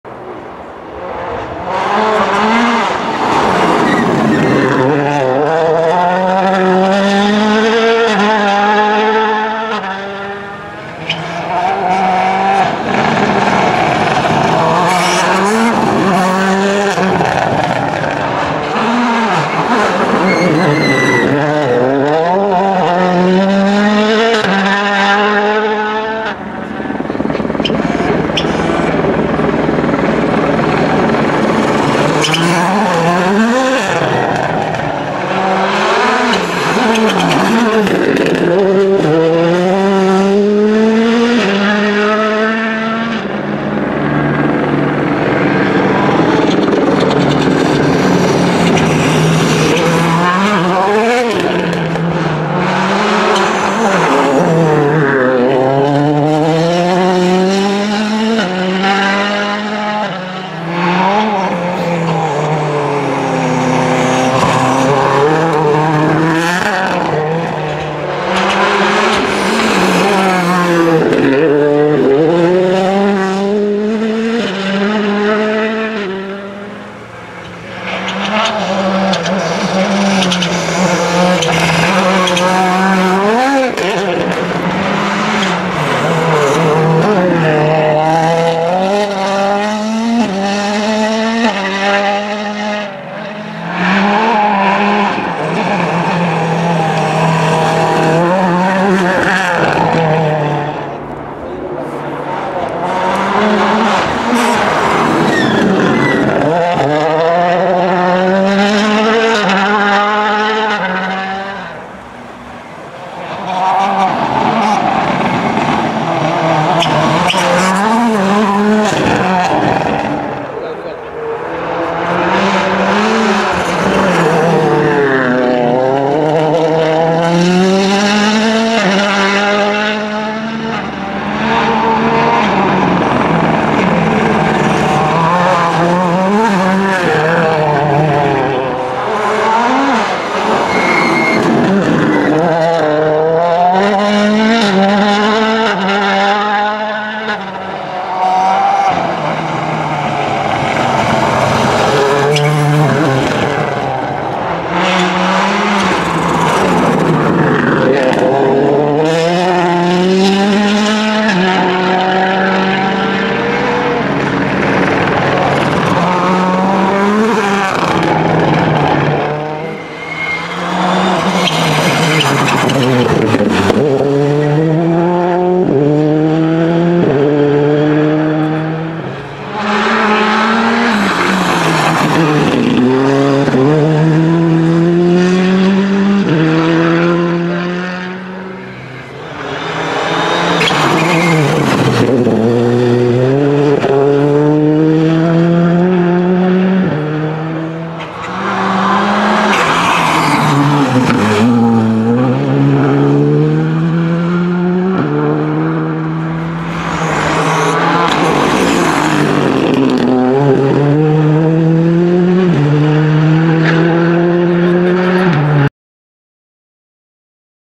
Rallye Monte Carlo 2020 - Day 3 - Sun and slush [HD]
Here we go for the last day of the monte for me ! Stage 10 and 12, La bréole - Selonnet, they were pretty much cautious on this one ! Enjoy !